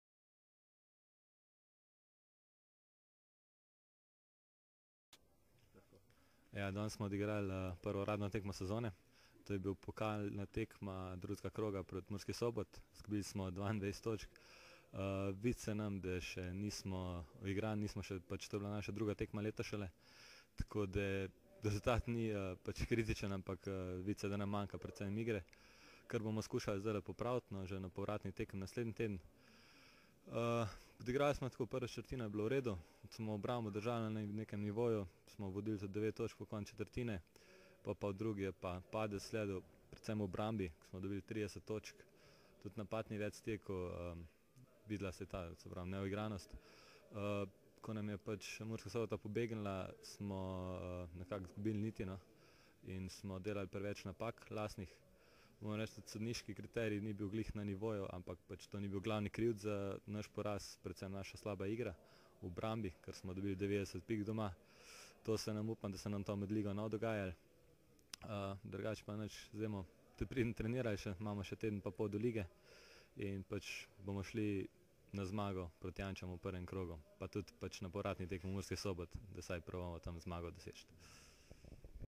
Pokal Spar: Črt Lipovšek
2. krog pokala Spar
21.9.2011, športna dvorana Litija
KK Litija : KK Radenska Creativ 67:89